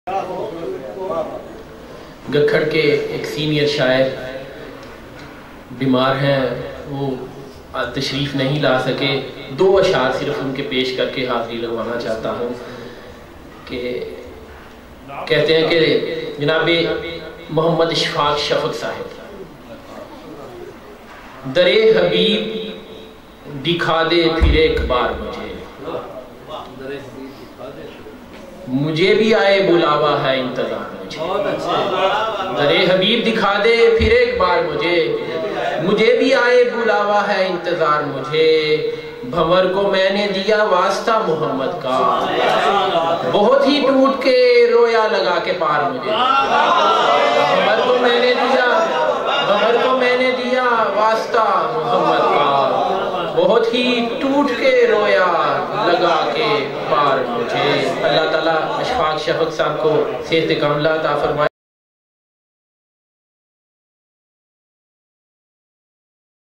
ashfaq shafaq
Aftab Ahmed is reciting Ashfaq Shafaq's Naat at Rida marriage lawns, inauguration ceremony of Collection of naats by naveed iqbal from Ghakhar mandi poets